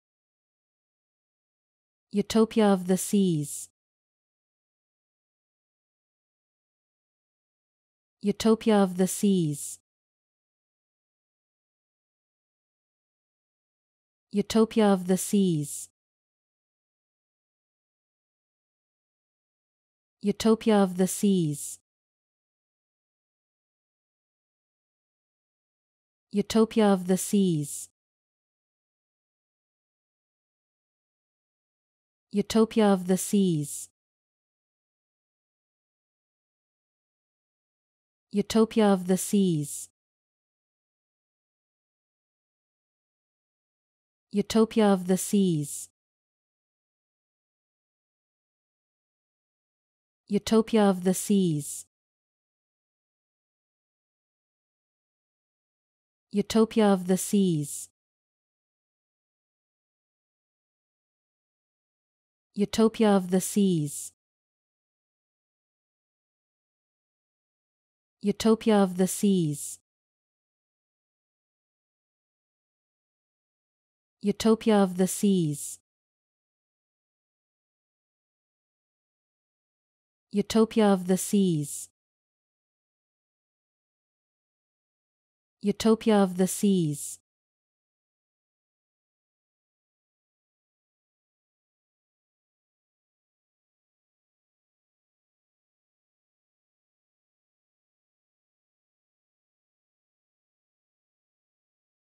How to Pronounce ''Utopia Of The Seas''
Listen to the pronunciation of ''Utopia Of The Seas'' and learn how to say it correctly in English. Browse EmmaSaying for more short pronunciation videos.